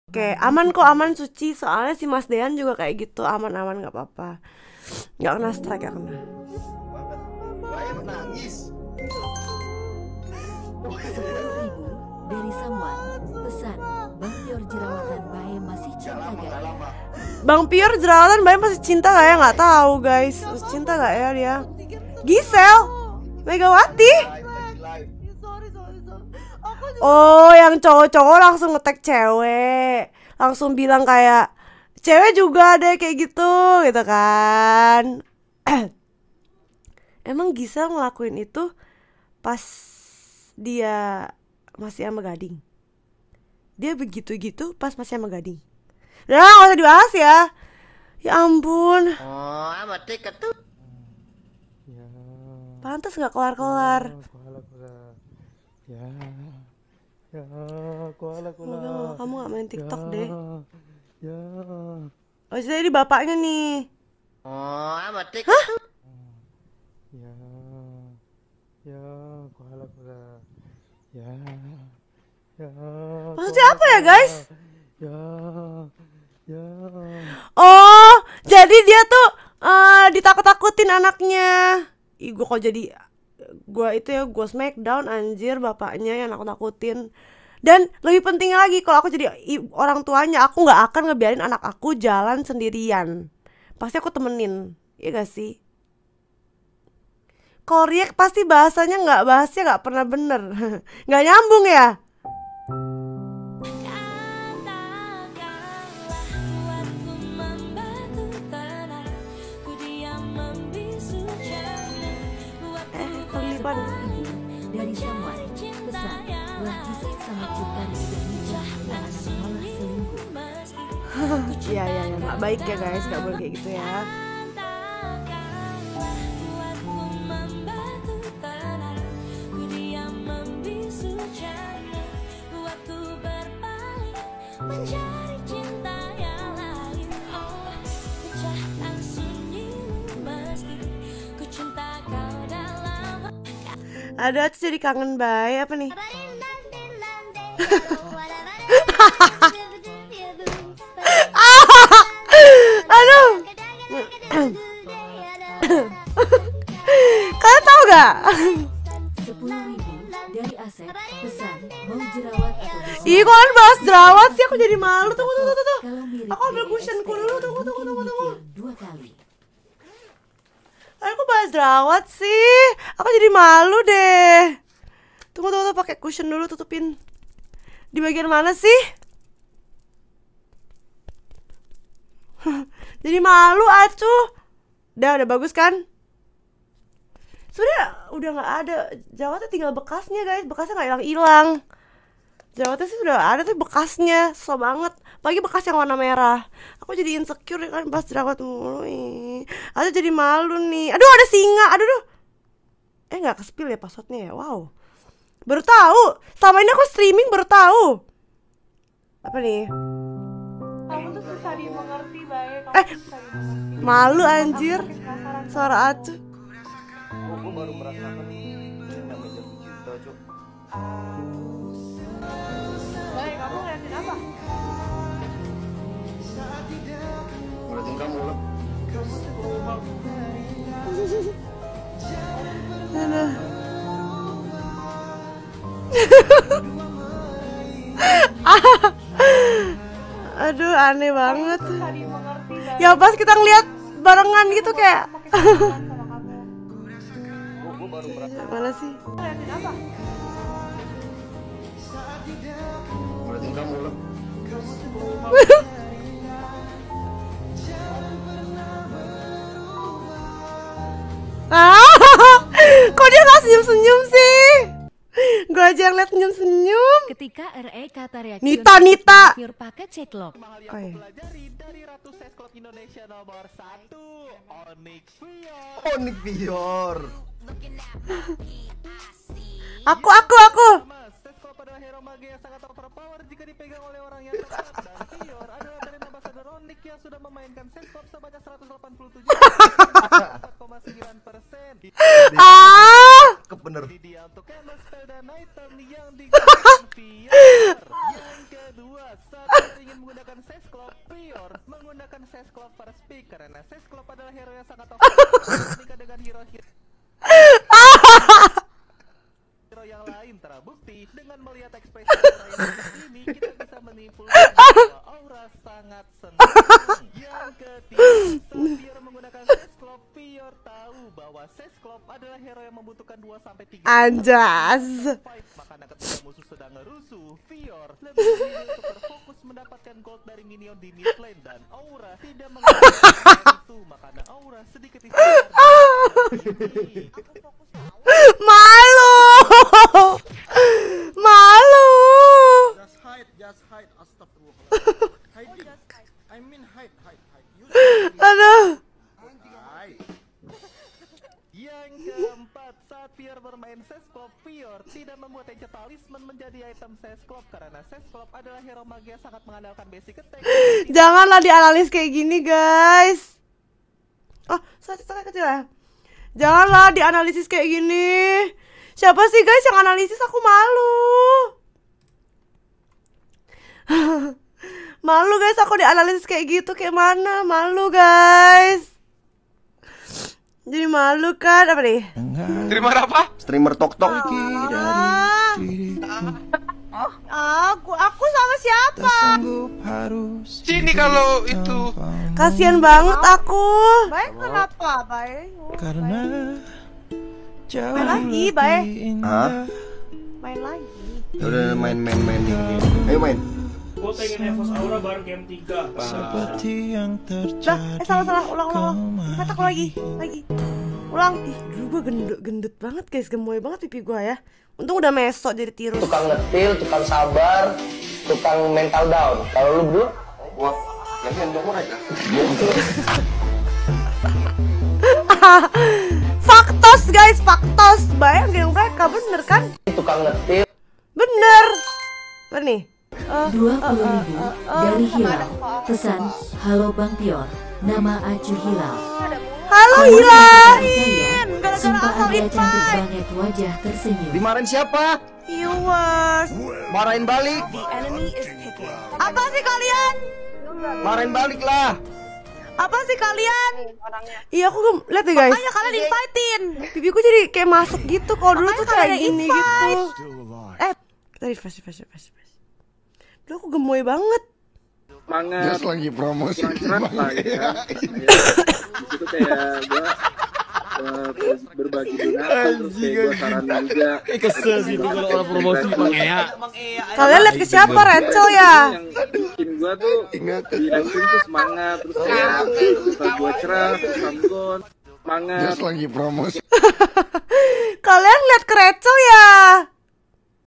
VIOR Ngakak React Videonya Lagi Liat Liatan Sama REKT - Kelucuan VIOR Insecure Karena Jerawat
VIOR NGAKAK REACT VIDEONYA LAGI LIAT LIATAN SAMA REKT
KELUCUAN VIOR INSECURE KARENA JERAWAT
LUCUNYA VIOR MALU SAAT LIAT REKT REACT TIKTOK VIOR